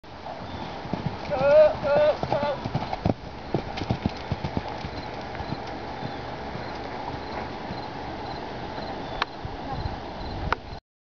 Logan free jumping 4'
My friend, Elaine free jumping Logan about 4' (I could be wrong). Enjoy! :)